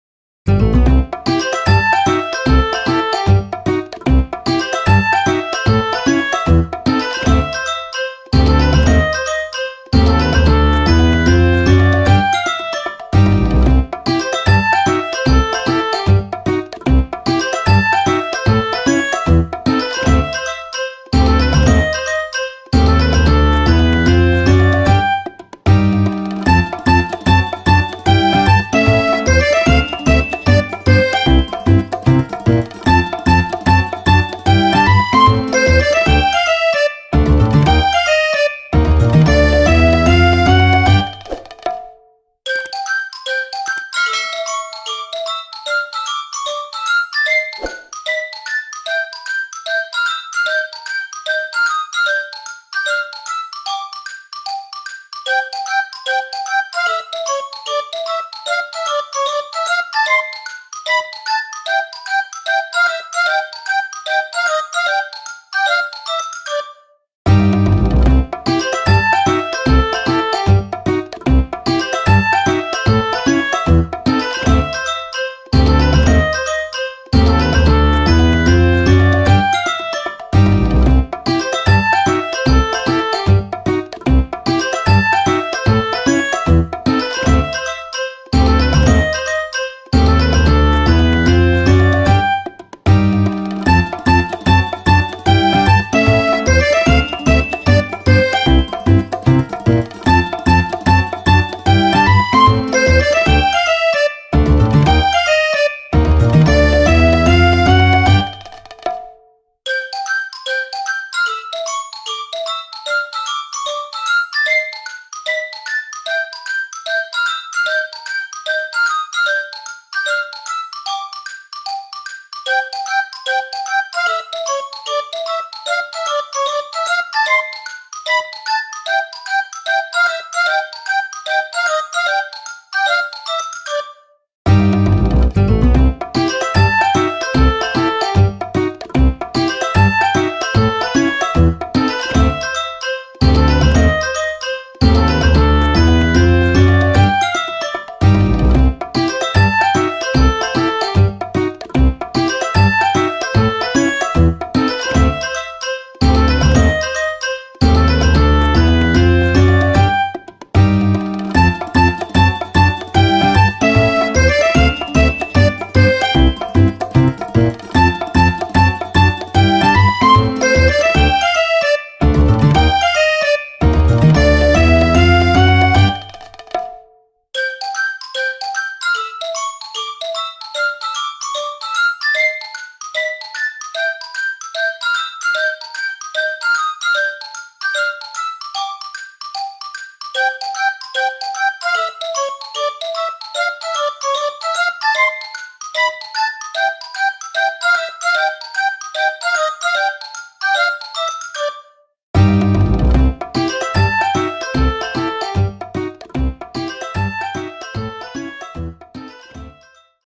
Kitty student Drawing, Painting & Coloring For Kids and Toddlers_ Child Art
Welcome to our art class, where today, our special student is a cute kitty! 🐱 She's ready to learn how to draw, paint, and color with all of you. Let's grab our art supplies and join her on this creative journey! 🎨 As we explore different colors and shapes, watch as our kitty student's imagination comes to life on the canvas. Together, we'll create beautiful masterpieces and learn new skills. Let's get started! 🖌️👫